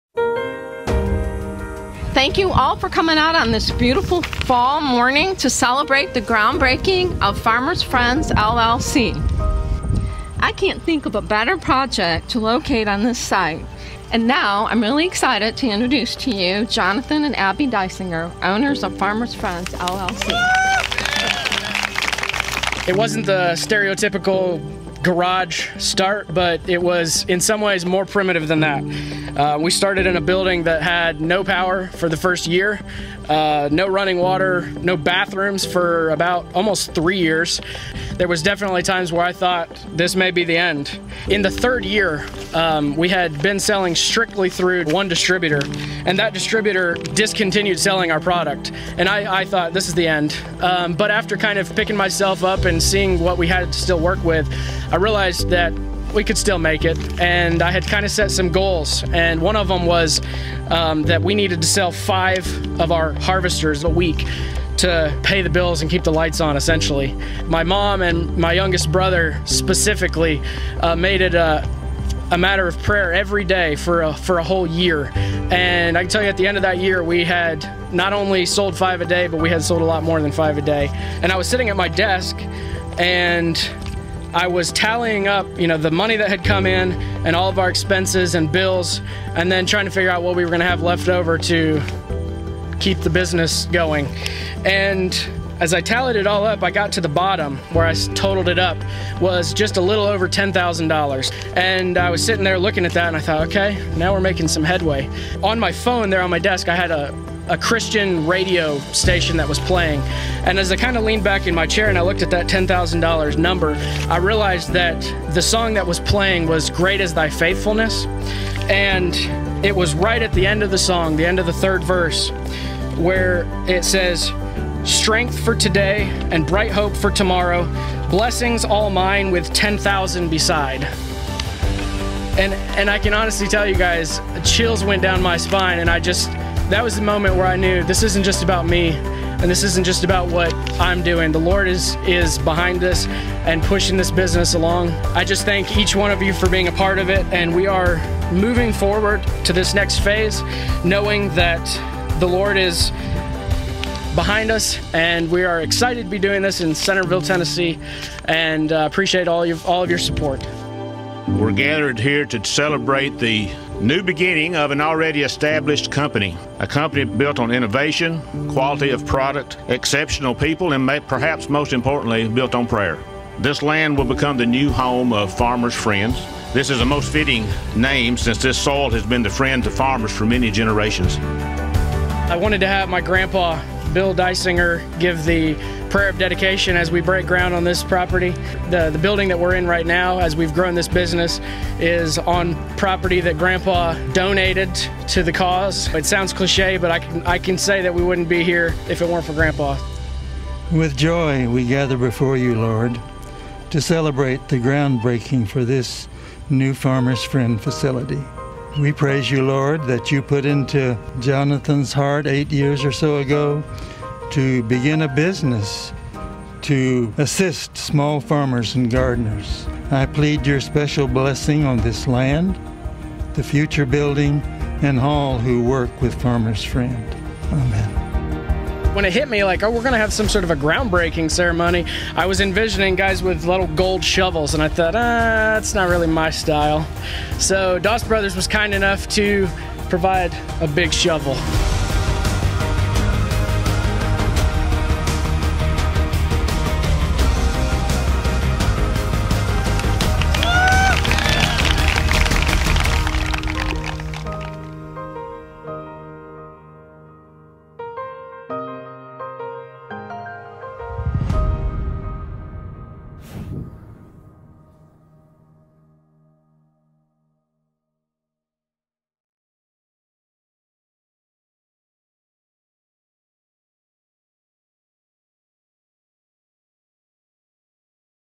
Farmers Friend Groundbreaking // Short Film
October 20, 2020 was an exciting day for Jonathan & Abby, and the whole Farmers Friend team, as we broke ground at the new property.

We have big plans for this land and new Farmers Friend facility, and are excited to see what the future holds.

Thank you for your support as our company grows and we look forward to serving you from this new location!

MB01RTAMZXAJQ0H